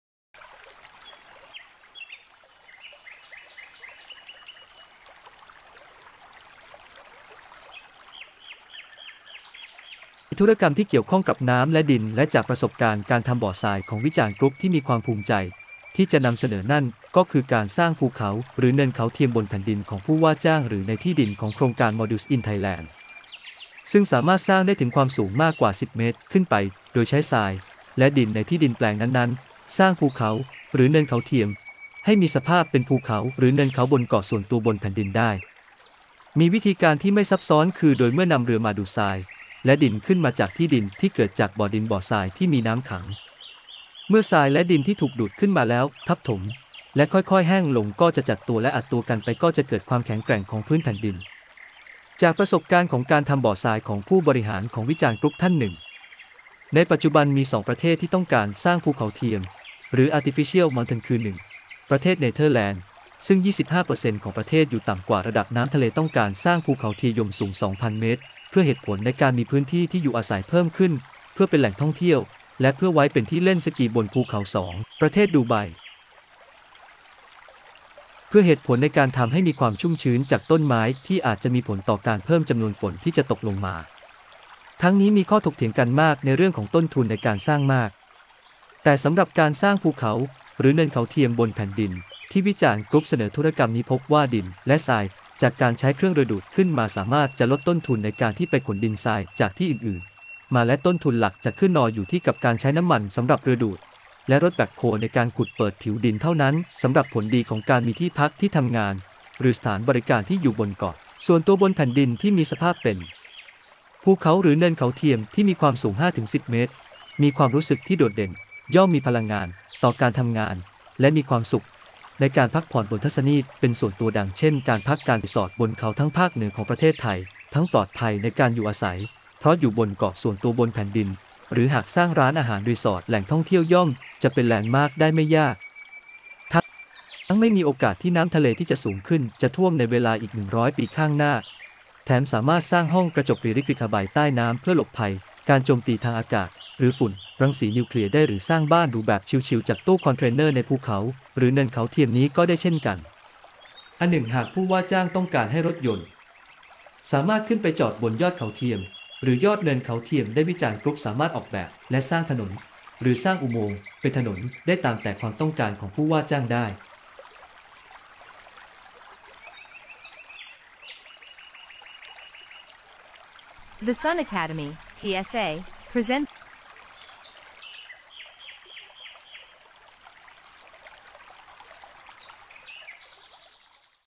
ภูเขาหรือเนินเขาเทียมบนเกาะส่วนตัวบนแผ่นดินของผู้ว่าจ้าง หรือในที่ดินของ โครงการMaldives In Thailand
ภูเขาหรือเนินเขาเทียมบนเกาะส่วนตัวบนแผ่นดินของผู้ว่าจ้าง หรือในที่ดินของ โครงการMaldives In Thailand โดย Vicharn Group ที่สูง 10 เมตร ขึ้นไป